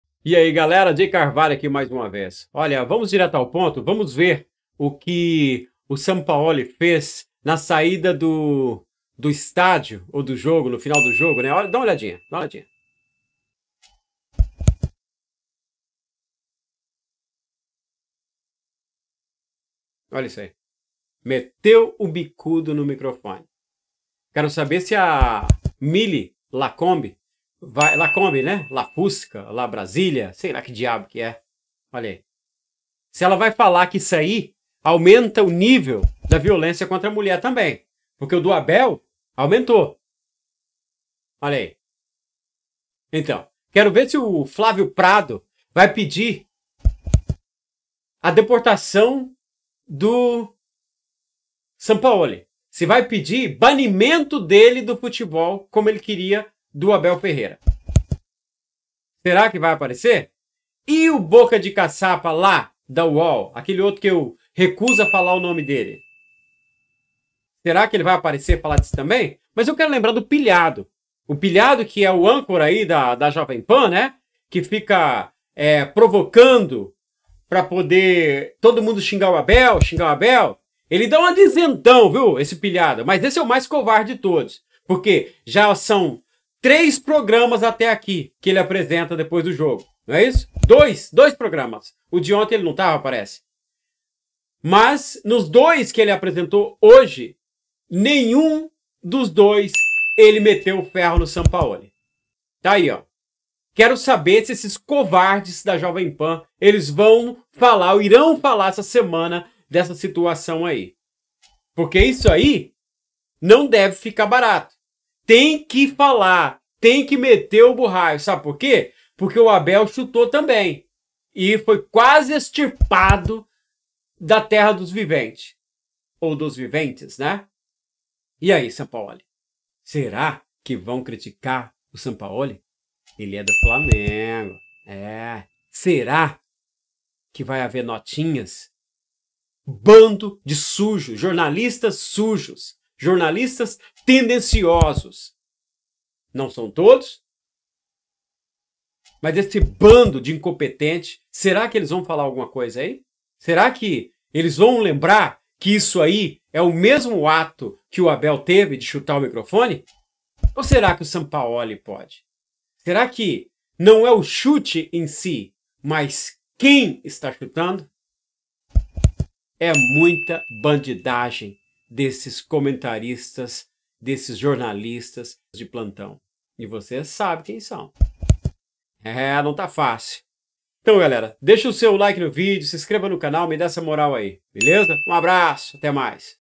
Sampaoli mete a bicuda no Microfone - imprensa caladinha - @pilhado acovardou
no jogo entre Flamengo e America MG, a técnico do flamengo Jorge Sampaoli sai no final do jogo cuspindo maribondos e deu bicudas no microfone perto da area tecnica.
a curiosidade e que os jornalistas que condenaram o Abel Ferreira quando chutou o mincrofone ainda nao falaram nada sobre o